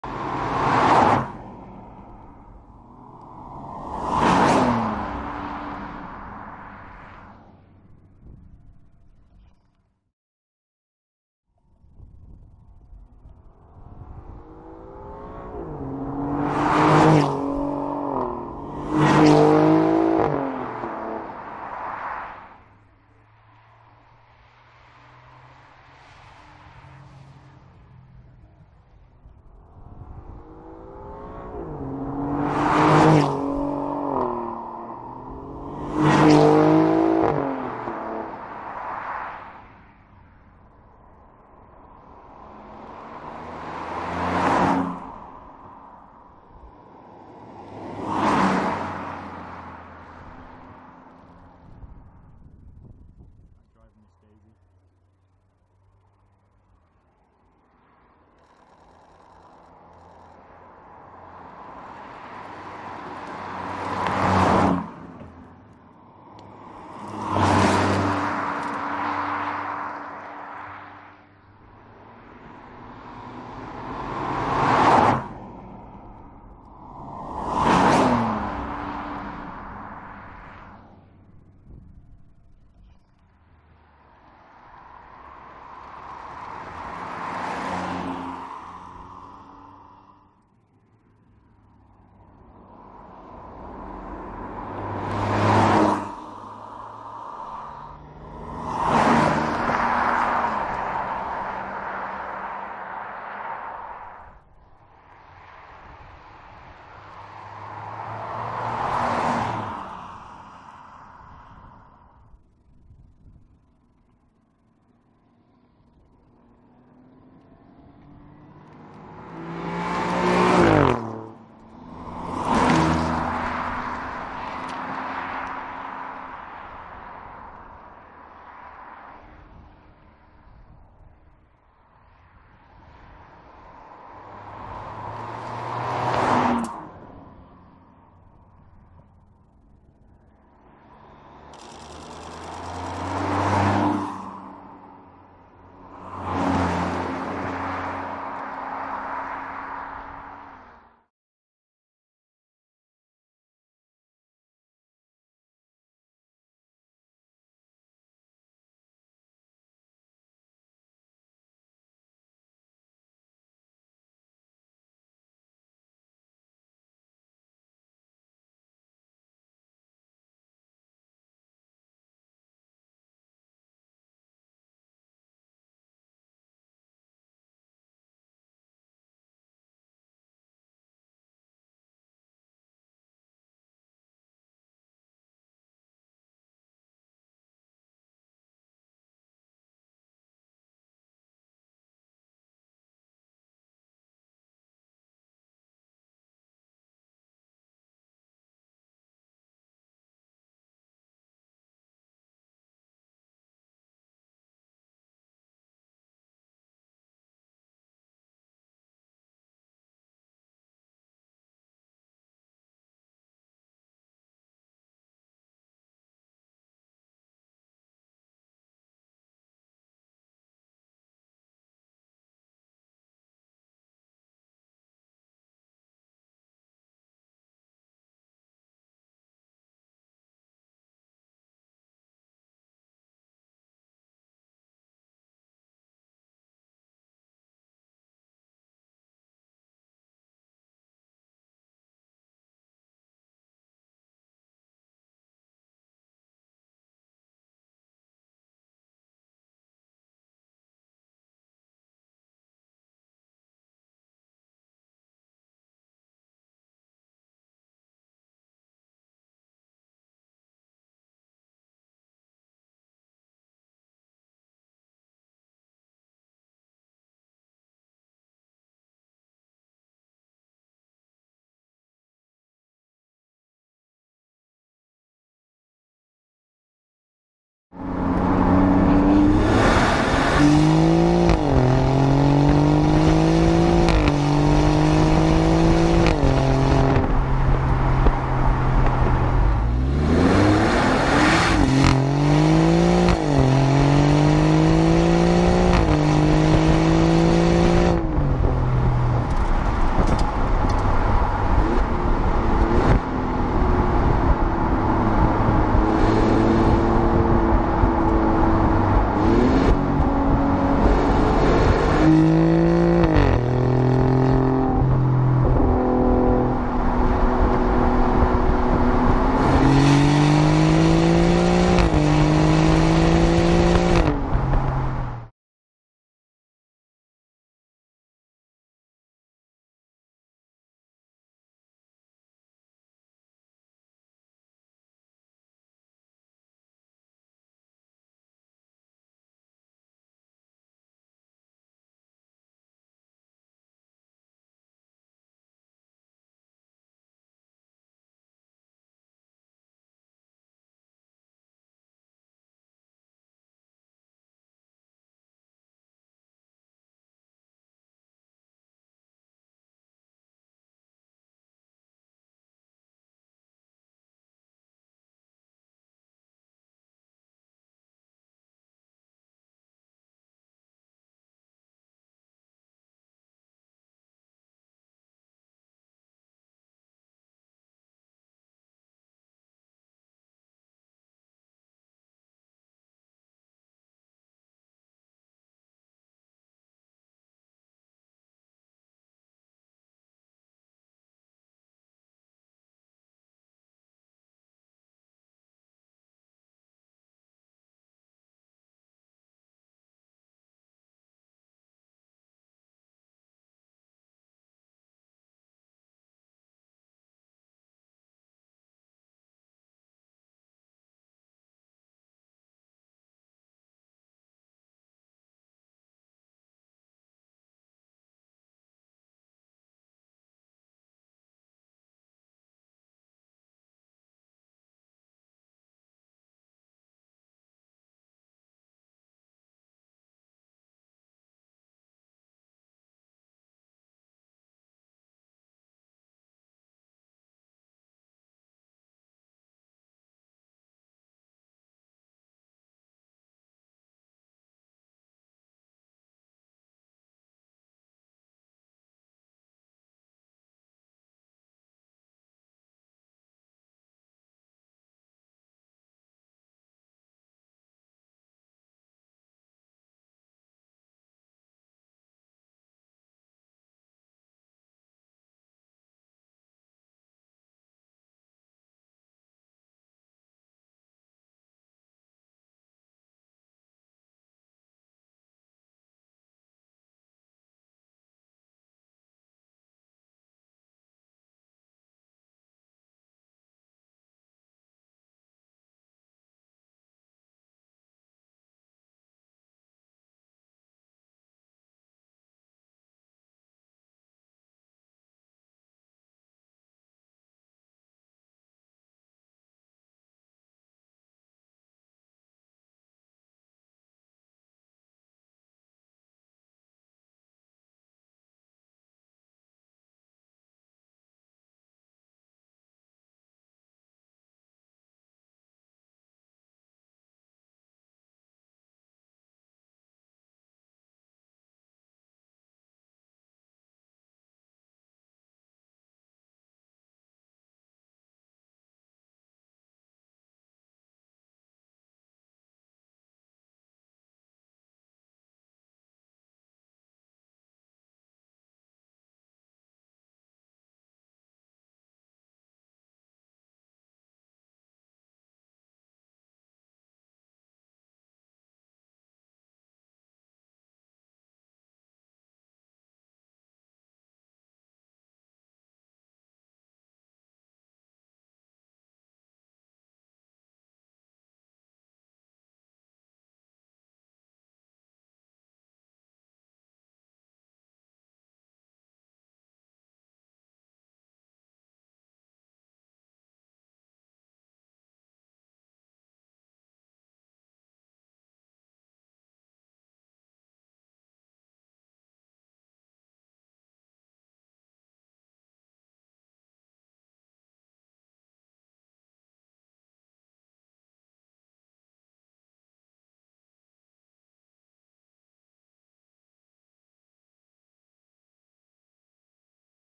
2020 Alfa Romeo Giulia Quadrifoglio Verde
A trembling finger prods the ‘stop’ button and a deafening silence floods the cabin. My ears are ringing, my wrists are numb and my forearms ache. Excess adrenalin turns my body into a sack of Maris Pipers. On the ragged edge of legal velocities deep in the South Downs National Park, I’ve just spent the final few hours of a great week with arguably the best Alfa Romeo ever made.